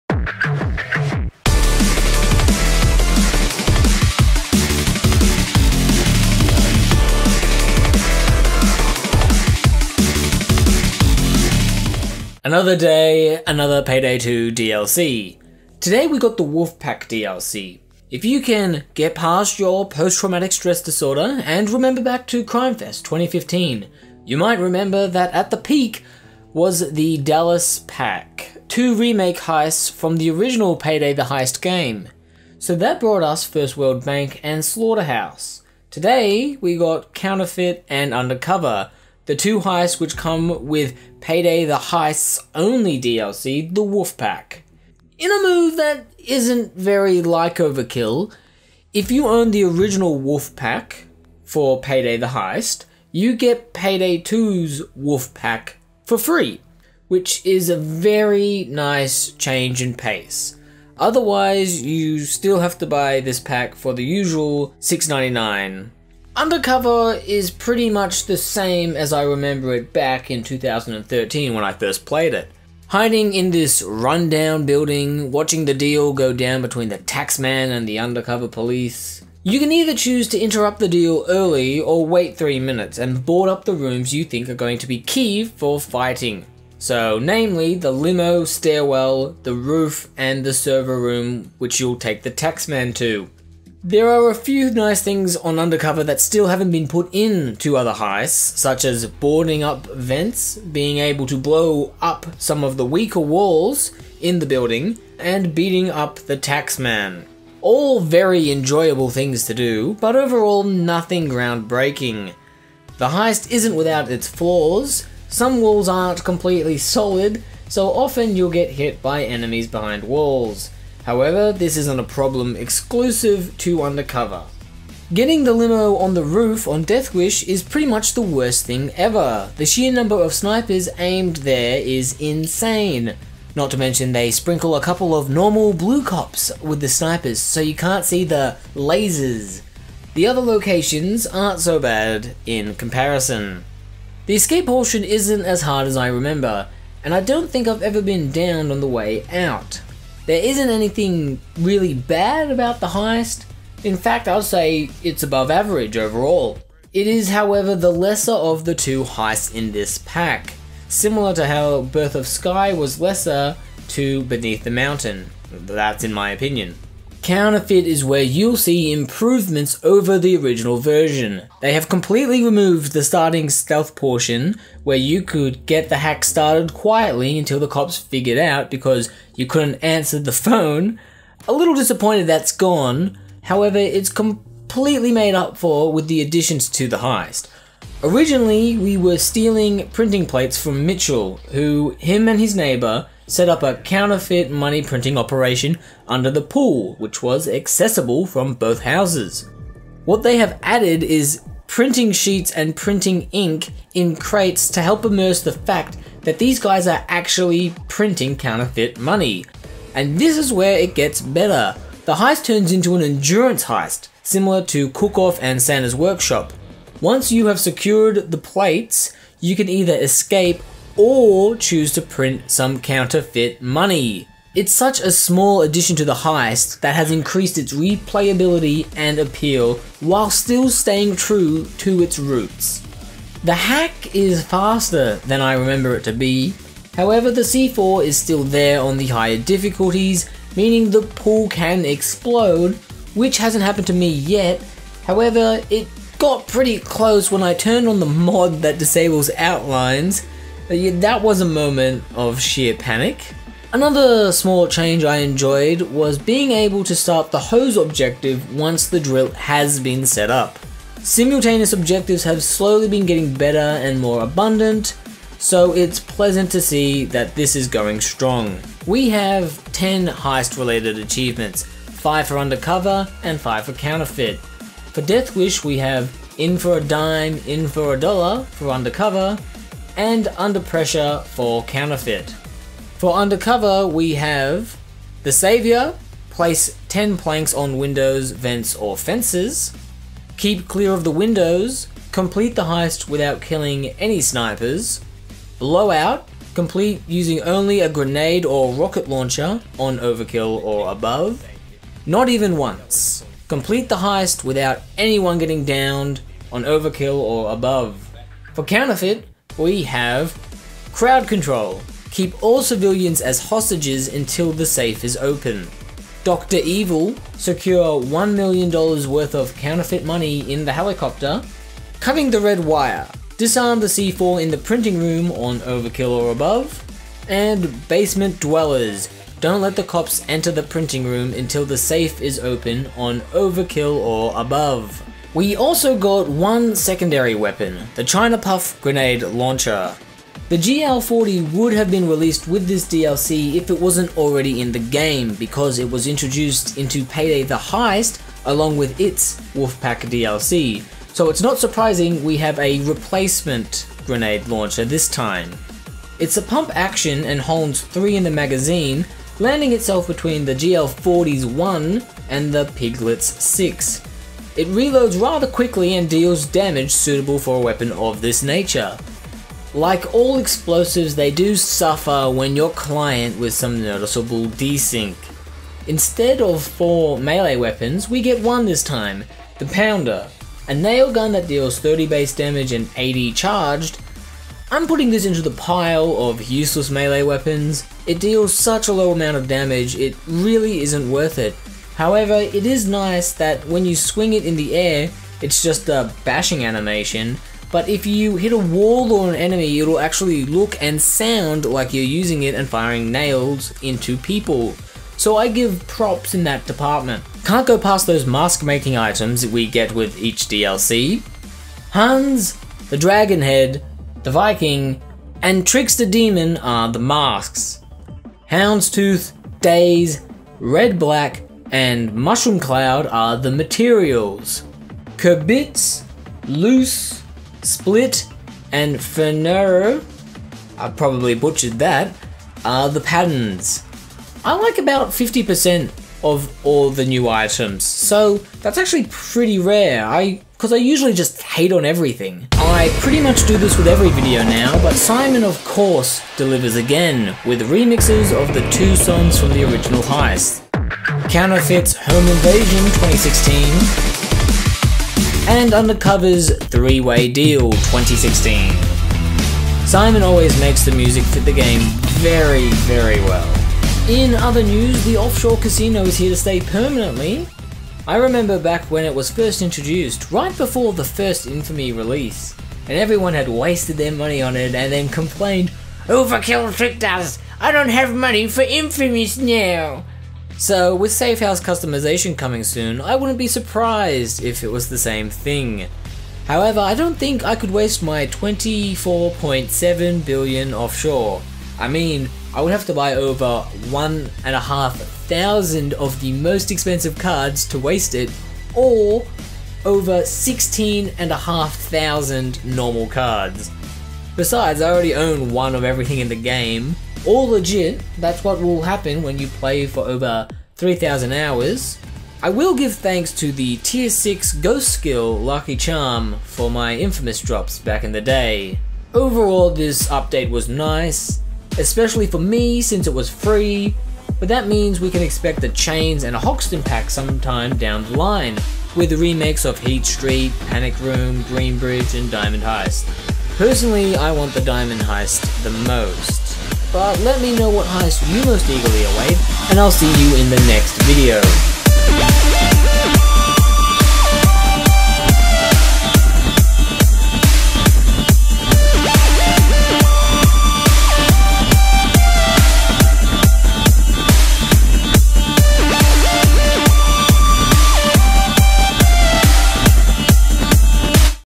Payday 2 - Wolf Pack Review
Hey I made it rather quickly this time

My Review of the recent Wolf Pack DLC for Payday 2